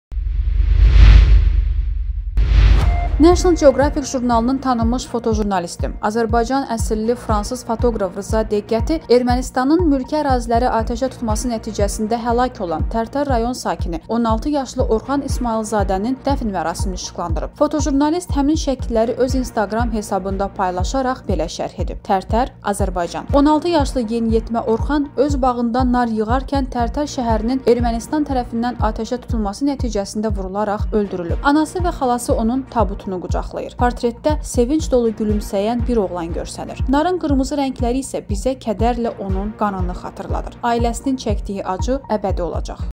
“Narın qırmızı rəngləri Orxanın qanını xatırladır”
“National Geografic” jurnalının tanınmış fotojurnalisti, Azərbaycan əsilli fransız fotoqraf Rza Diqqəti Ermənistanın mülki əraziləri atəşə tutması nəticəsində həlak olan Tərtər rayon sakini 16 yaşlı Orxan İsmayılzadənin dəfn mərasimini işıqlandırıb.
Fotojurnalist həmin şəkilləri öz instaqram hesabında paylaşaraq belə şərh verib:
“Tərtər, Azərbaycan.
16 yaşlı yeniyetmə Orxan öz bağında nar yığarkən Tərtər şəhərinin Ermənistan tərəfindən atəşə tutulması nəticəsində vurularaq öldürülüb.
Anası və xalası onun tabutunu qucaqlayır. Portretində sevinc dolu gülümsəyən bir oğlan görsənir. Narın qırmızı rəngləri isə bizə kədərlə onun qanını xatırladır. Ailəsinin çəkdiyi acı əbədi olacaq”.